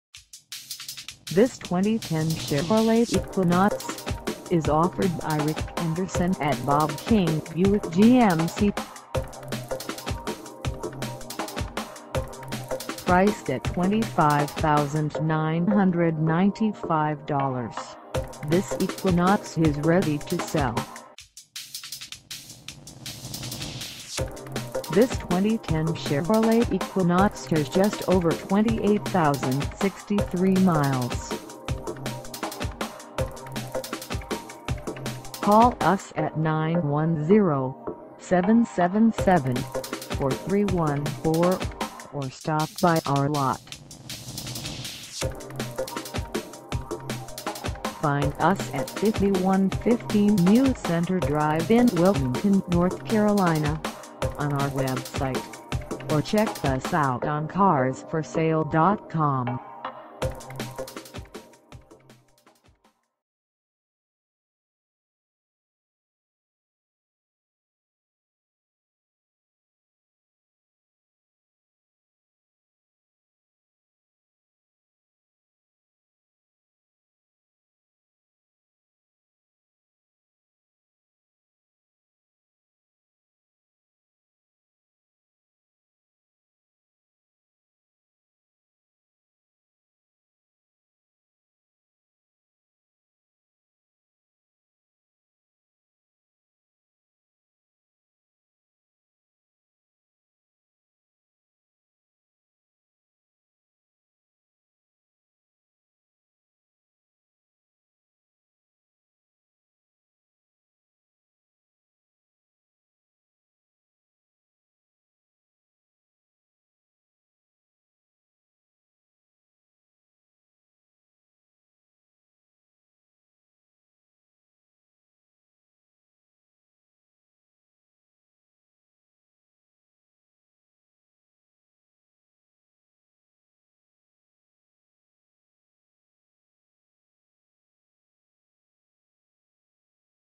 2010 Chevrolet Equinox LT - for sale in Wilmington, NC 28403
5115 NEW CENTRE DR

Come test dirve this 2010 Chevrolet Equinox LT for sale in Wilmington, NC.